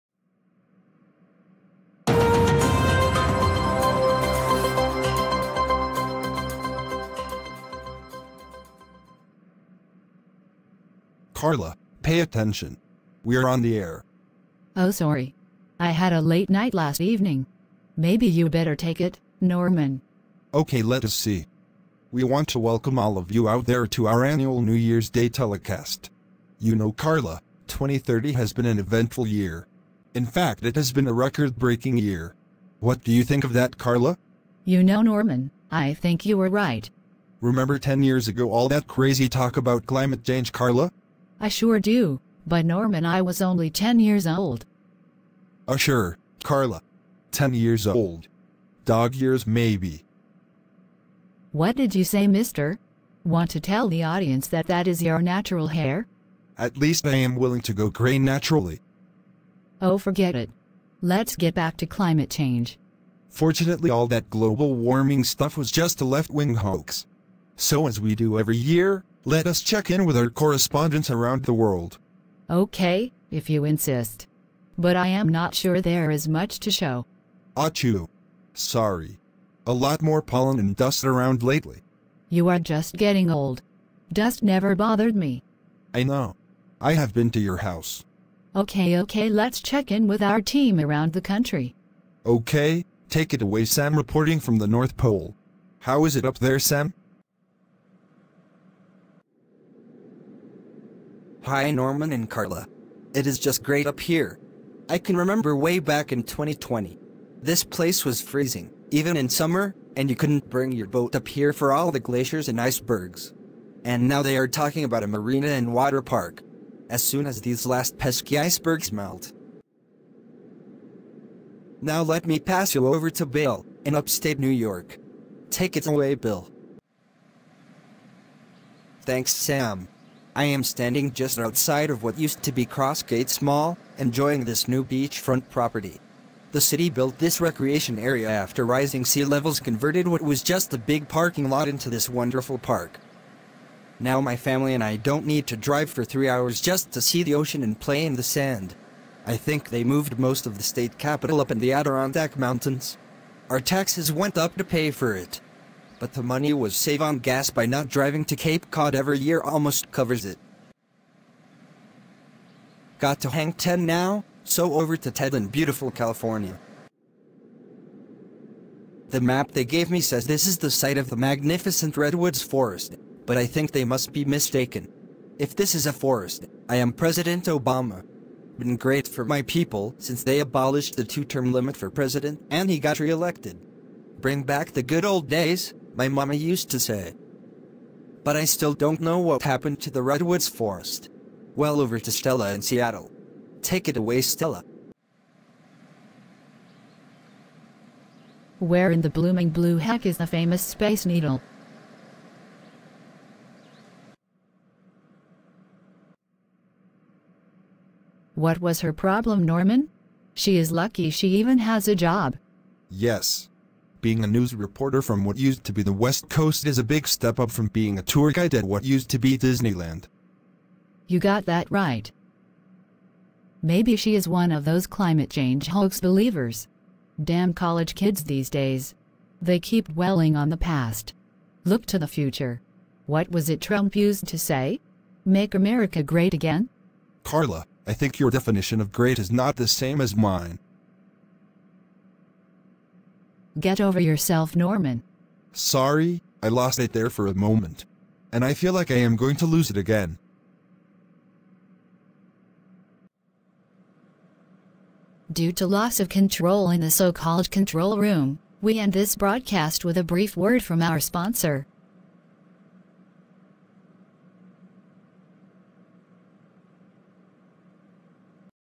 Climate Change 2030
A spoof on climate change as it is viewed in the future.